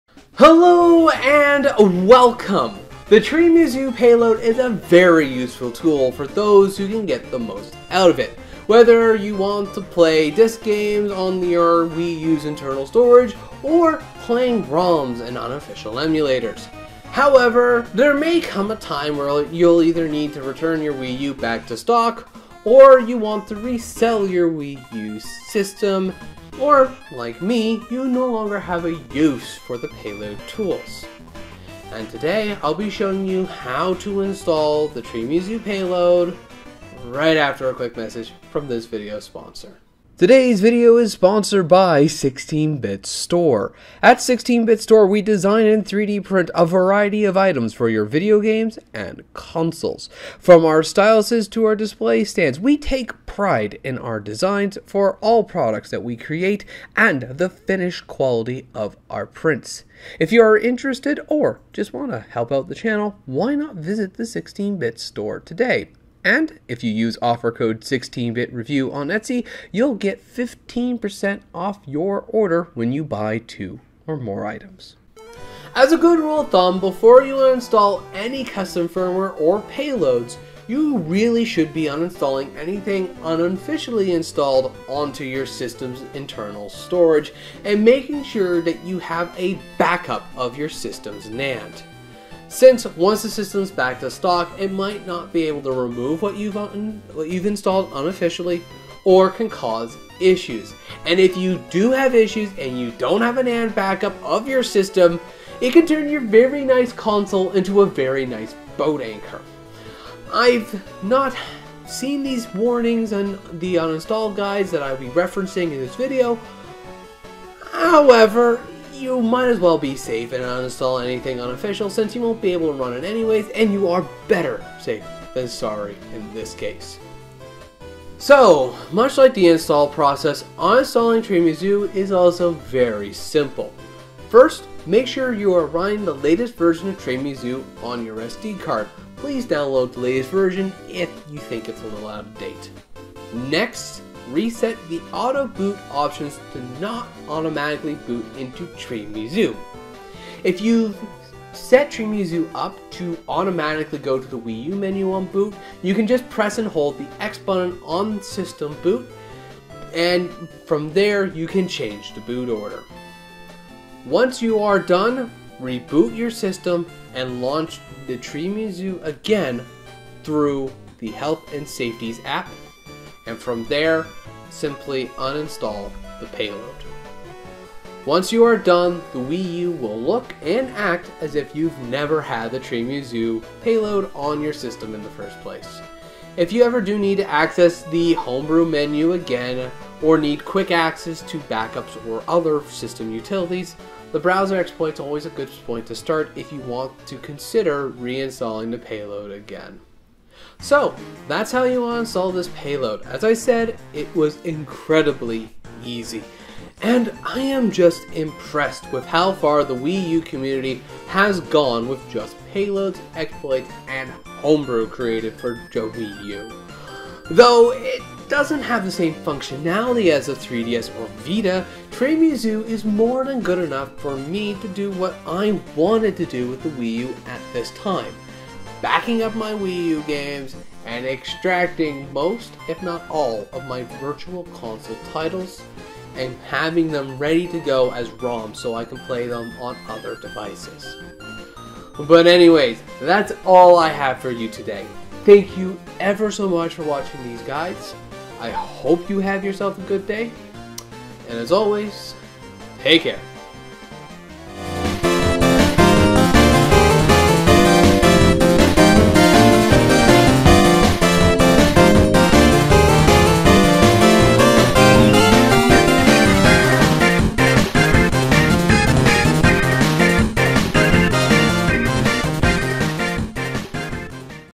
How to Uninstall Tiramisu Payload - 16 Bit Guide
In this video I will show you how to uninstall the Tiramisu Payload from your Wii U, and summarize my experience with it.